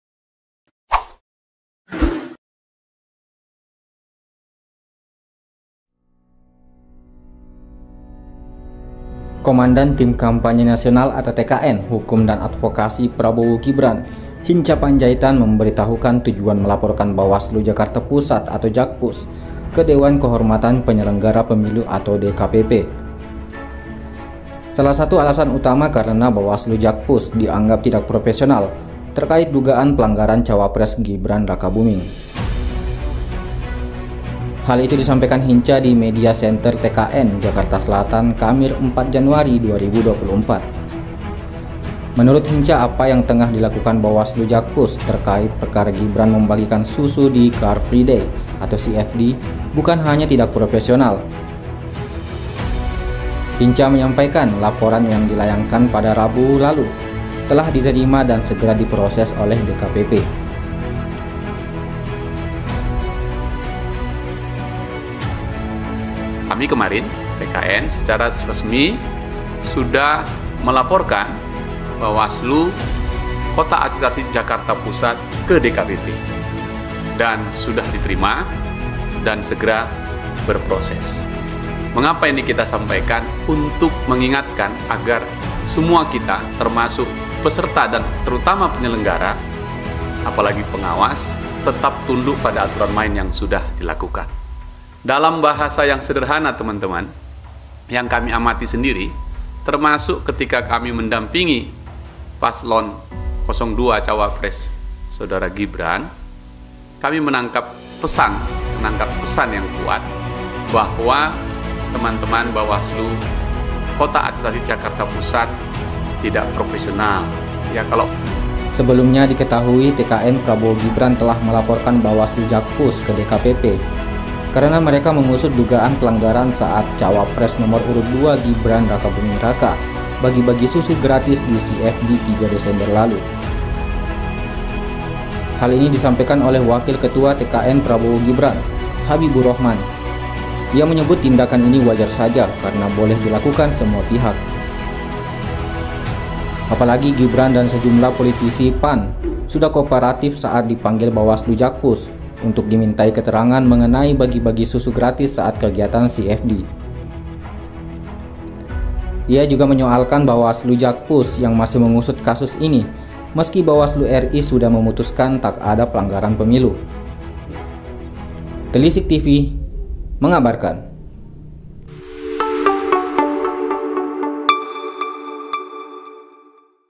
TKN Prabowo-Gibran Beberkan Alasan Laporkan Bawaslu Jakpus ke DKPP
Komandan Tim Kampanye Nasional (TKN) Hukum dan Advokasi (Echo) Prabowo-Gibran, Hinca Pandjaitan memberitahukan tujuan melaporkan Bawaslu Jakarta Pusat (Jakpus) ke Dewan Kehormatan Penyelenggara Pemilu (DKPP).

Salah satu alasan utama karena Bawaslu Jakpus dianggap tidak profesional terkait dugaan pelanggaran cawapres Gibran Rakabuming.